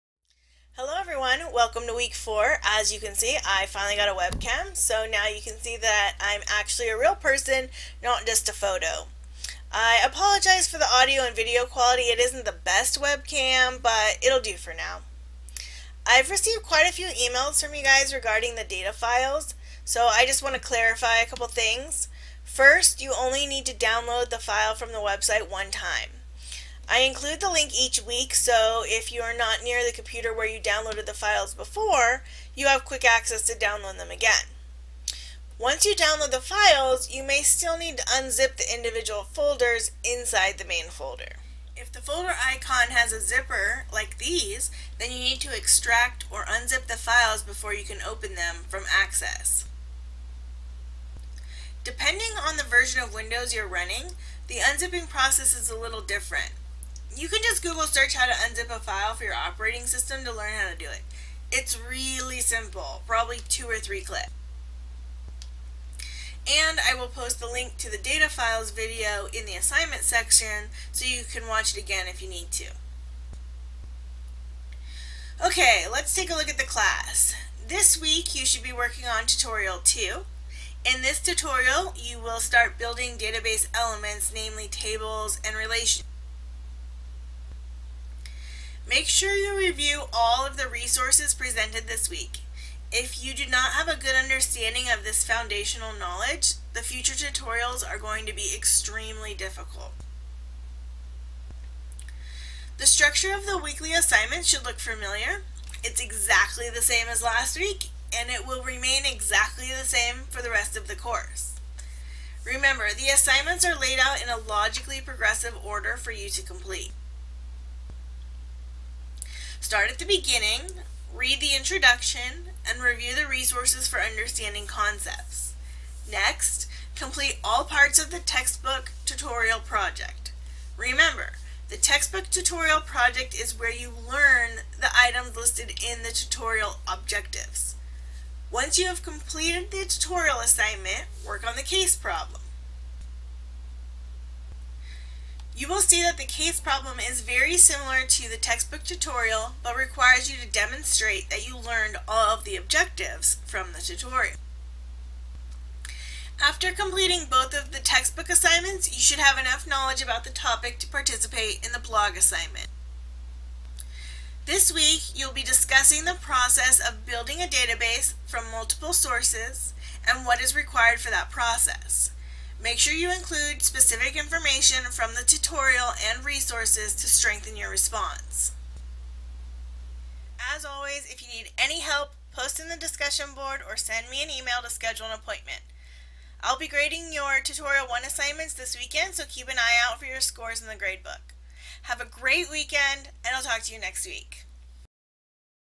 CAPP123 - Week Four (Fall 2013)
This video is an overview of how to complete the assignments in week four of the Fall 2013 MSJC CAPP123: Using Microsoft Access course.  I created this video with the YouTube Video Editor